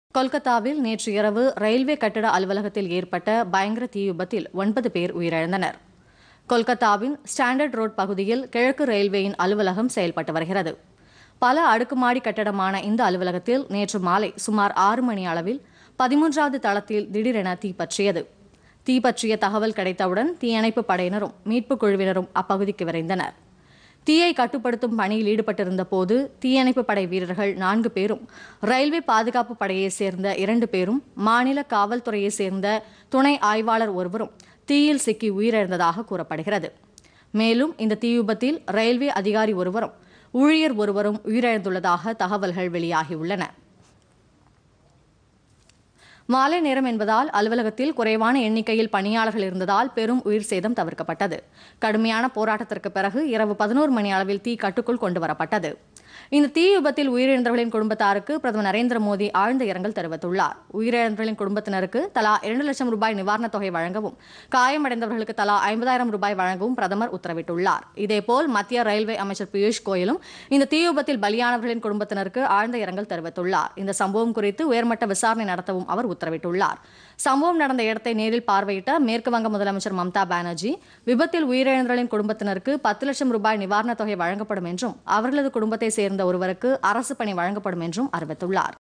கொல்கத்தாவில் ரயில்வே கட்டிட தீவிபத்து- 9 பேர் பலி
கொல்கத்தாவில் ரயில்வே கட்டிடத்தில் ஏற்பட்ட பயங்கர  தீவிபத்தில் 9 பேர் உயிரிழப்பு - உயர்மட்ட விசாரணைக்கு ரயில்வே அமைச்சர் பியூஷ் கோயல் உத்தரவு.